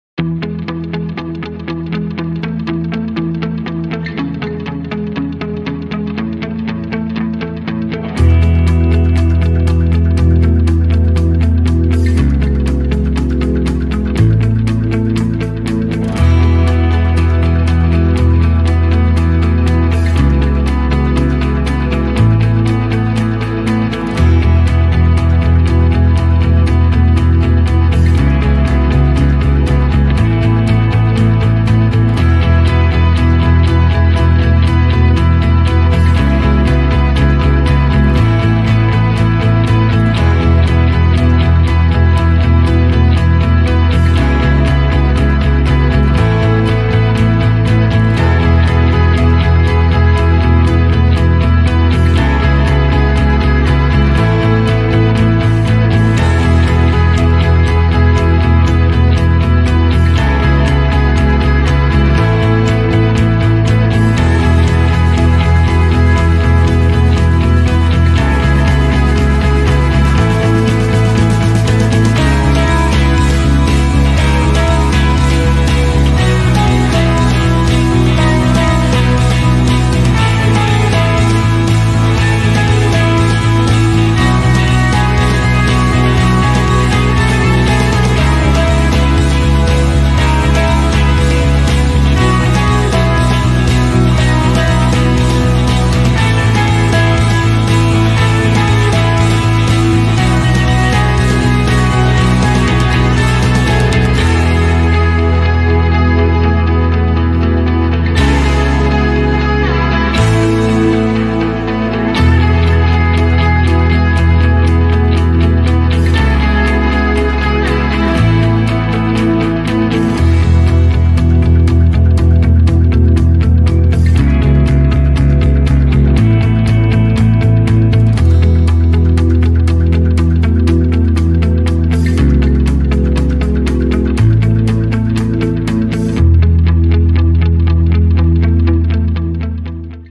District 58 staff share their
During the Jan. 4 Institute Day, Superintendent Dr. Kevin Russell challenged District 58 staff to select one word to focus on in 2021. Here are some highlights from District 58's OneWord2021 selections!